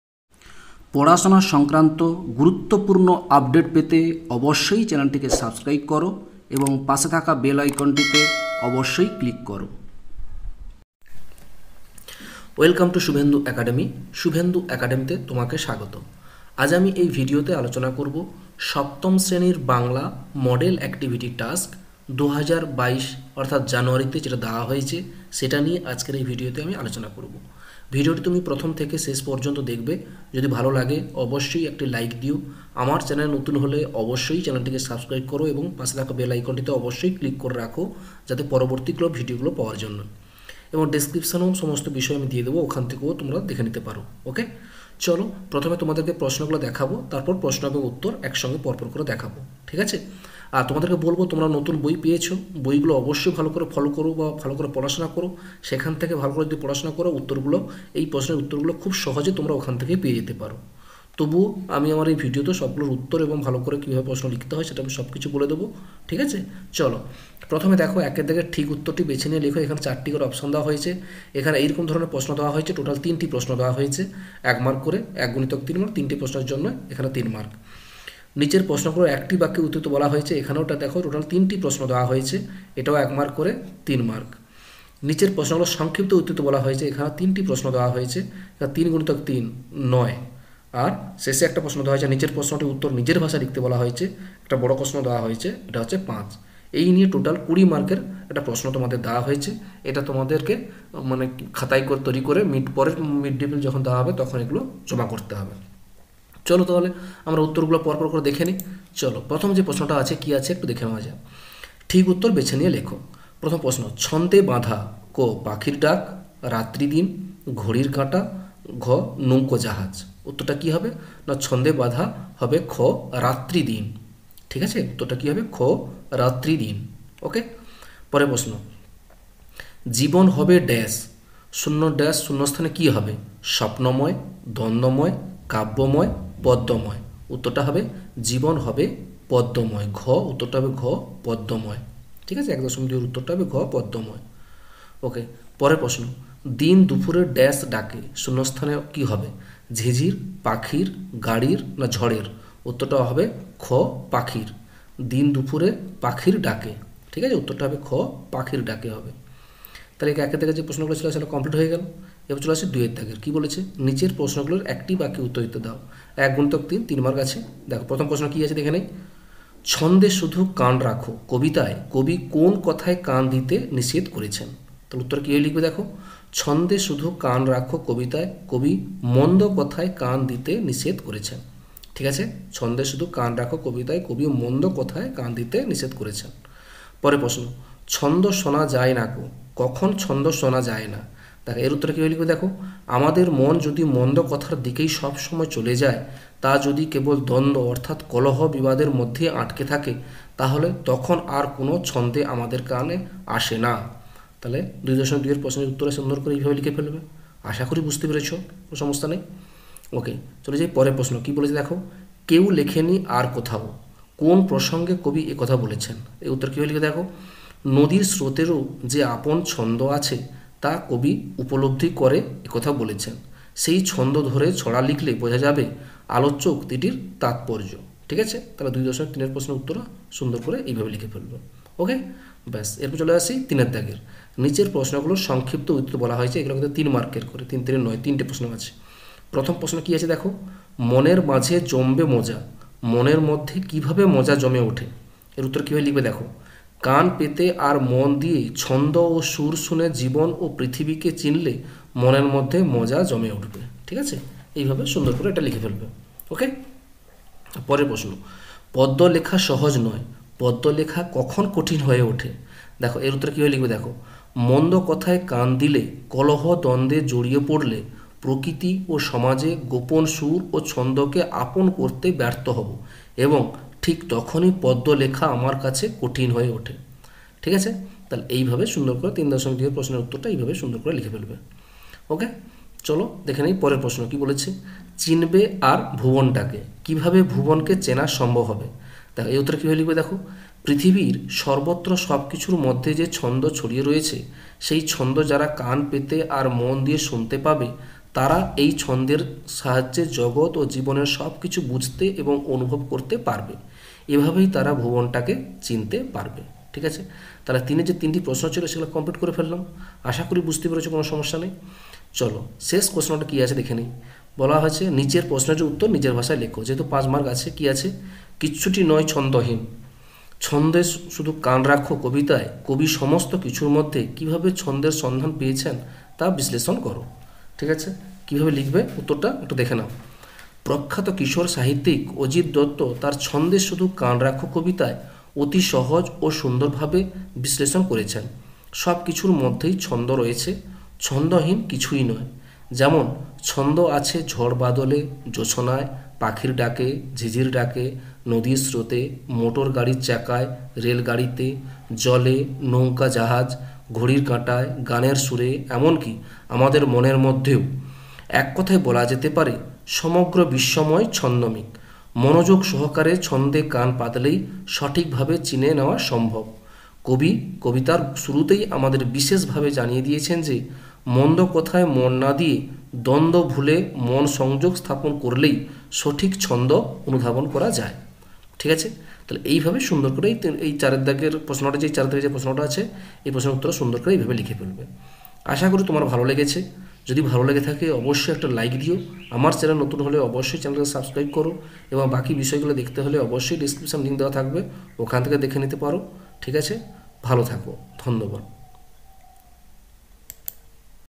Model Activity Task Class 7 Bangla (বাংলা) // January 2022 // Class VII Bengali Activity Task Part 1
Model Activity Task Class 7 Bangla (বাংলা) // January 2022 // Class VII Bengali Activity Task Part 1 //জানুয়ারি মাসের সপ্তম শ্রেণির বাংলা মডেল অ্যাক্টিভিটি টাস্ক এর প্রশ্ন এবং উত্তর// Class 7 Bangla January 2022 Model Activity Task Part 1 With Solution